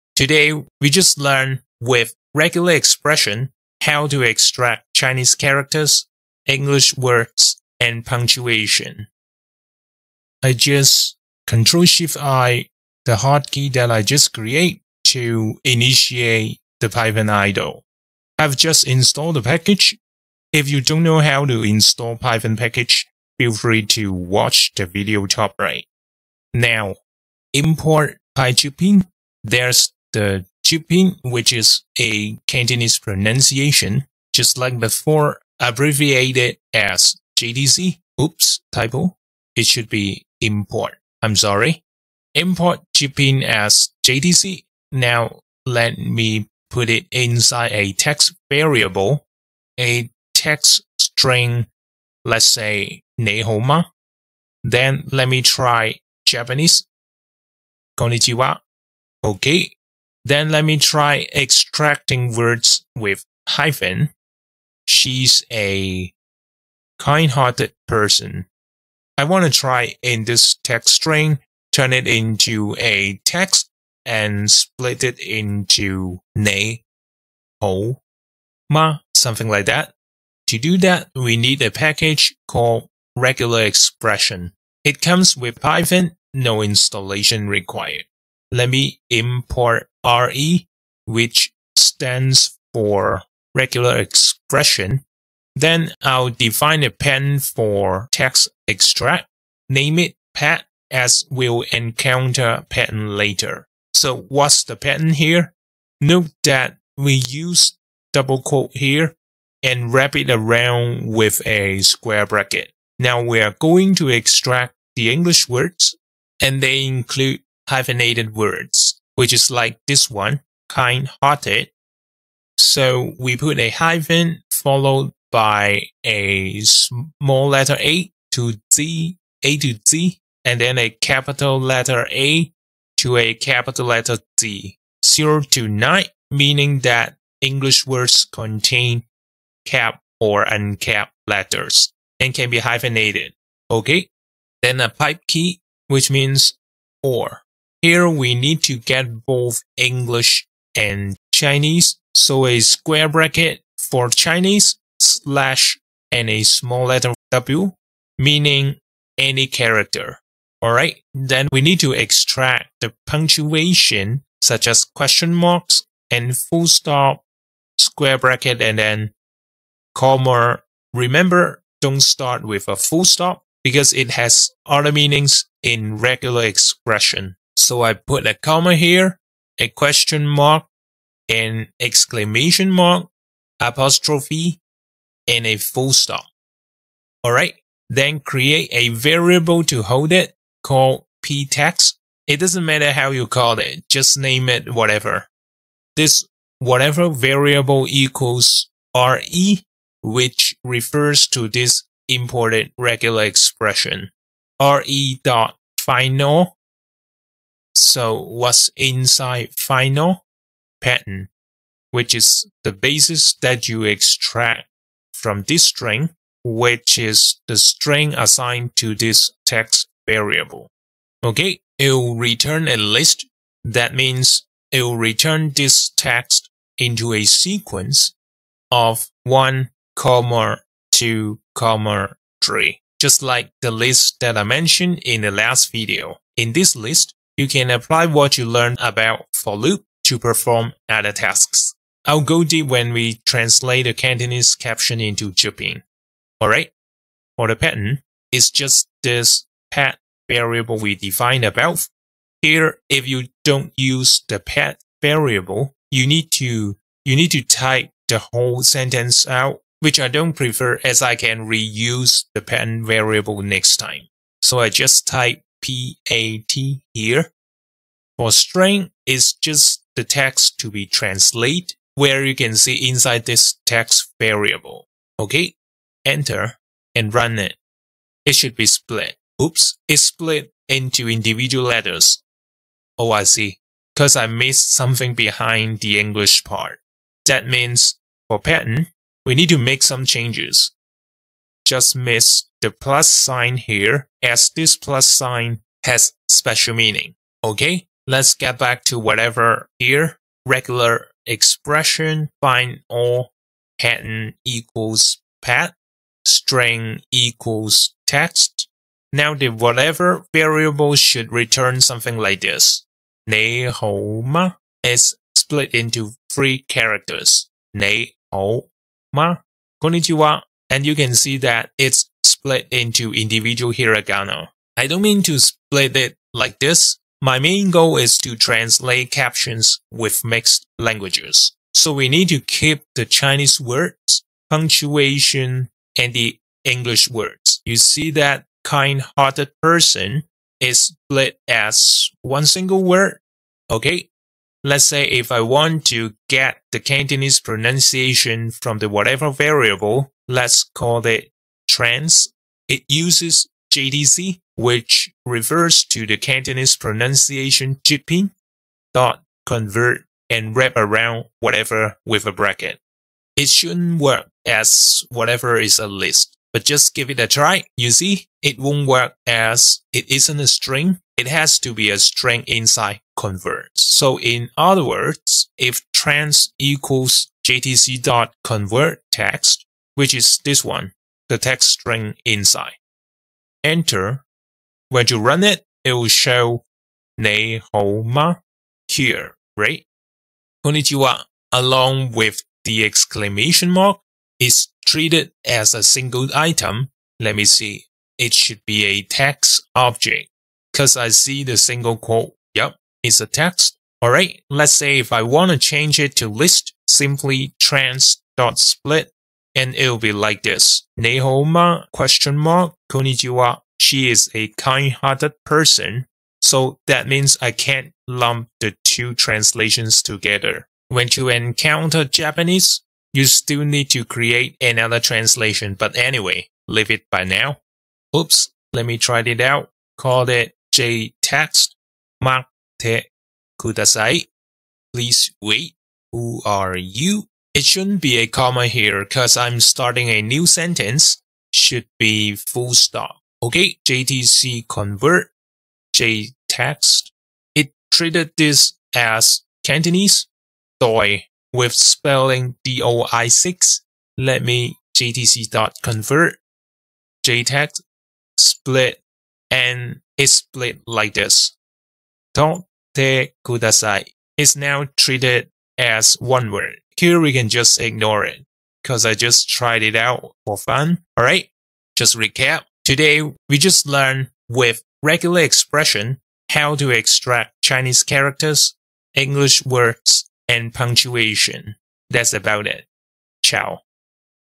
Extract Both English & Cantonese With Regular Expression
Learn how to extract Cantonese characters and English words using basic Python regular expression